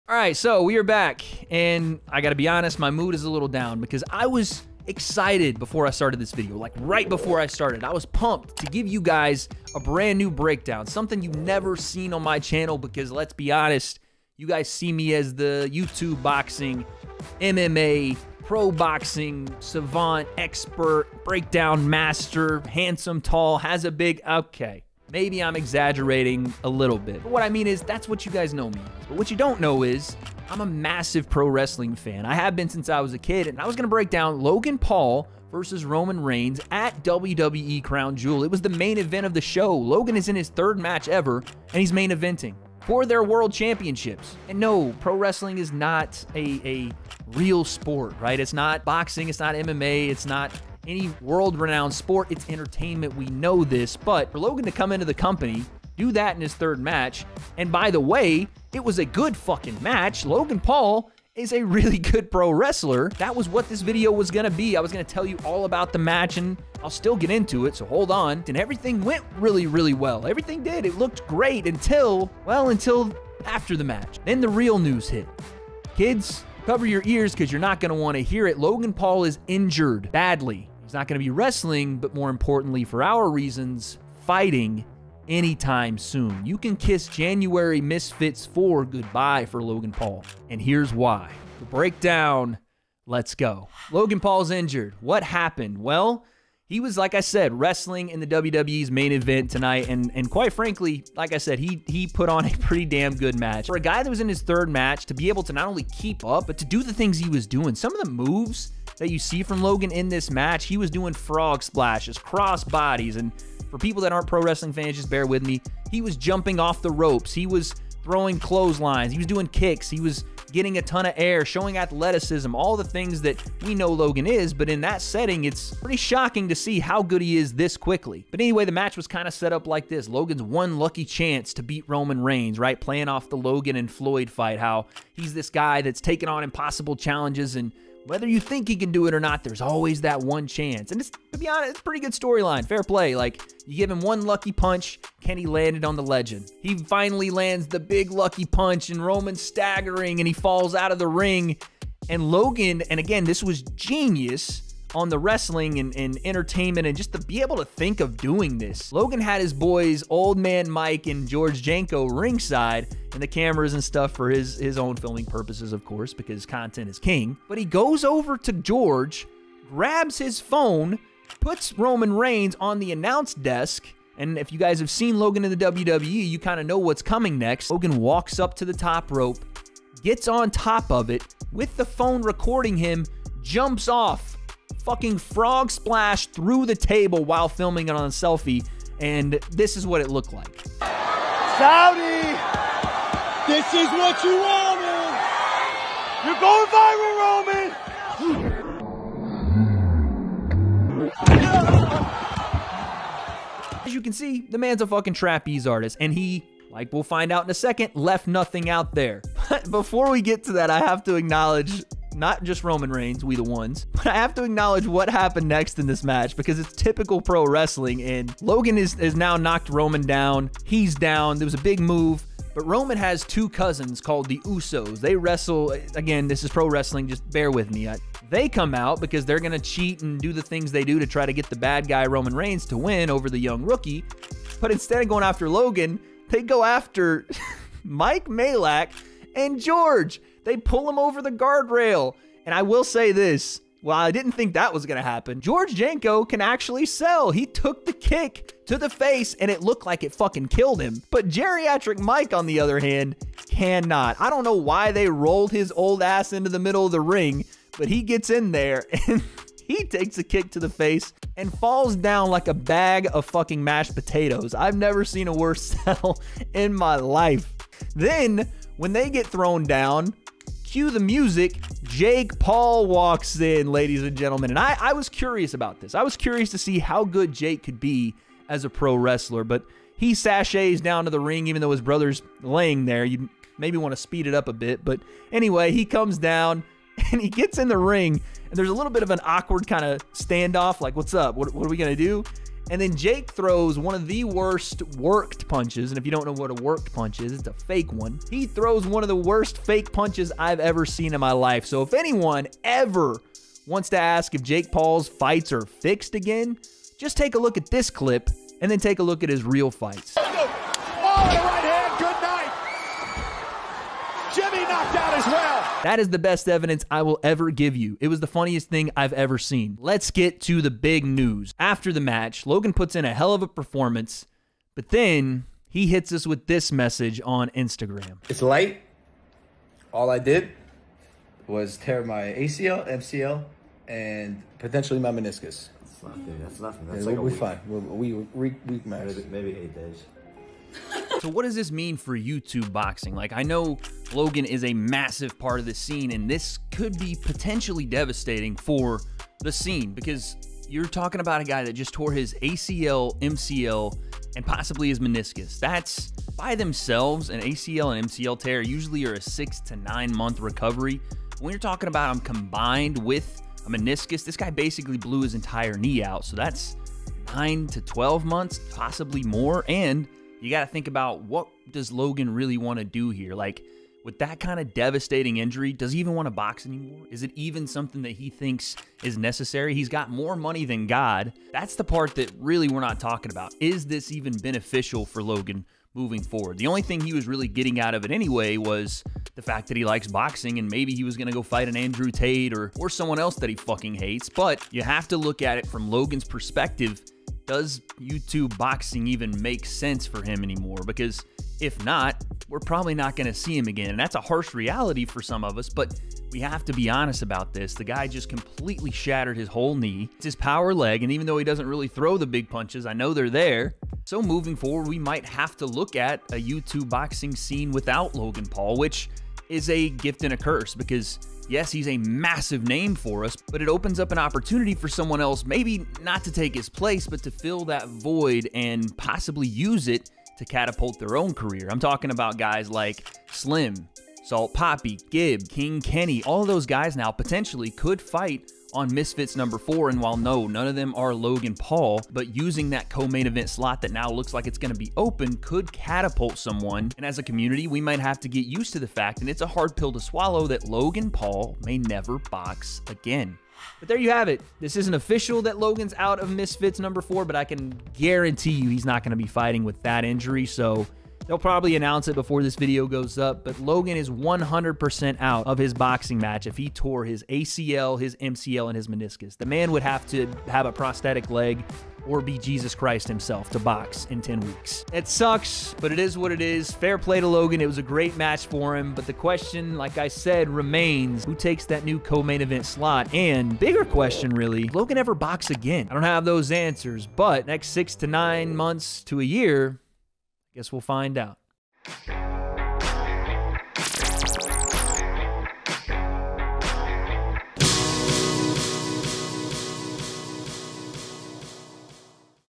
Why Logan Paul Will NEVER FIGHT Again..
Logan Paul's WWE match with Roman Reigns Just ended his boxing career.. Do you agree? Let's Debate!!

💥 FOLLOW THE W.A.D.E. Concept on Twitch!!

"The WADE Concept" 27758 Santa Margarita Pkwy Box 581 Mission Viejo, CA 92691